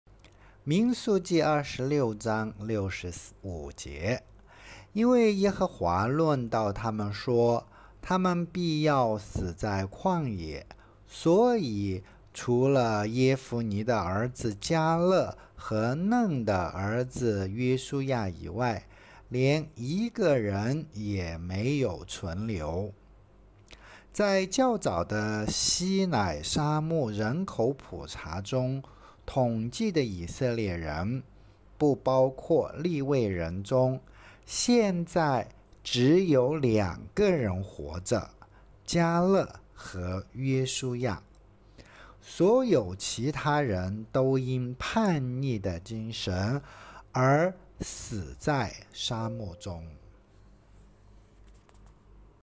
每日分享 三百九十三
民数记 二十六65 因为耶和华论到他们说：「他们必要死在旷野。」所以，除了耶孚尼的儿子迦勒和嫩的儿子约书亚以外，连一个人也没有存留。
在较早的西奈沙漠人口普查中统计的以色列人（不包括利未人）中，现在只有两个人活着，迦勒。 . .和约书亚。所有其他人都因叛逆的精神而死在沙漠中。
Bilangan 26:65 sebab TUHAN telah berfirman tentang mereka: "Pastilah mereka mati di padang gurun." Dari mereka itu tidak ada seorangpun yang masih tinggal hidup selain dari Kaleb bin Yefune dan Yosua bin Nun.
Dari orang Israel (tidak termasuk orang Lewi) yang dihitung dalam sensus Sinai sebelumnya, hanya dua yang sekarang hidup, Kaleb. ... dan Yoshua. Semua yang lain mati di gurun karena semangat memberontak mereka.